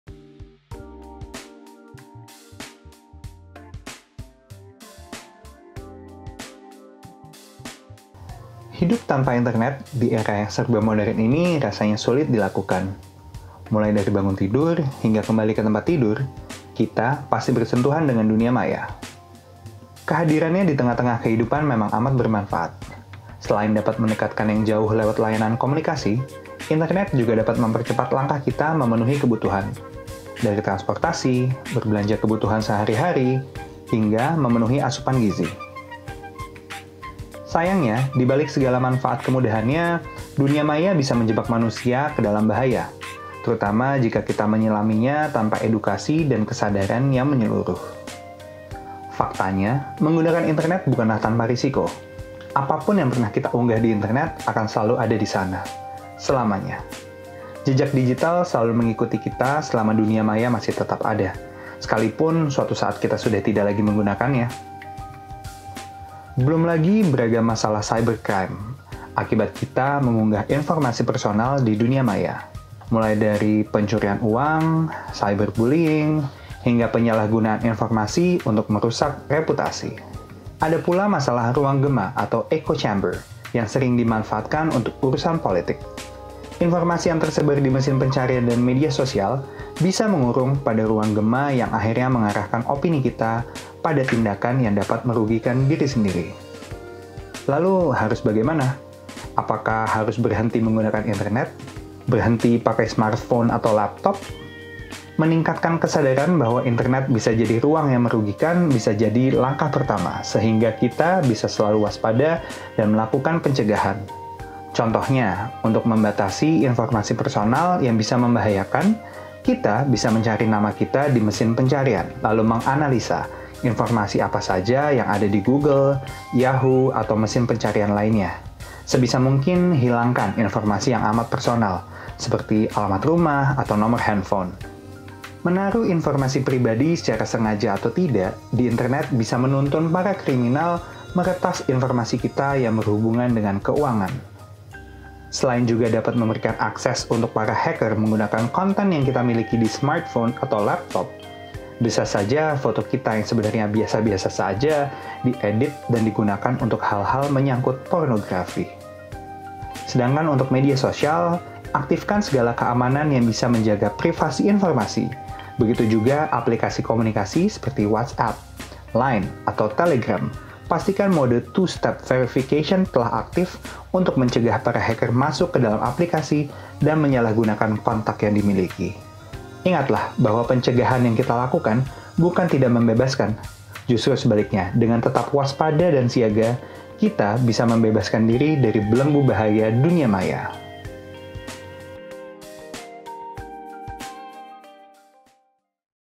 Rethinking Digital: Realita Dunia Maya
Hidup tanpa internet di era yang serba modern ini rasanya sulit dilakukan. Kehadirannya memang amat bermanfaat. Sayangnya, di balik segala kemudahannya, dunia maya bisa menjebak manusia ke dalam bahaya. Terutama jika kita menyelaminya tanpa edukasi dan kesadaran yang menyeluruh.

Rethinking Digital merupakan program terbaru GREATMIND yang bekerjasama dengan Disrupto sebuah platform inovasi teknologi dan sains, yang akan memberikan perspektif baru atas hubungan manusia dengan teknologi.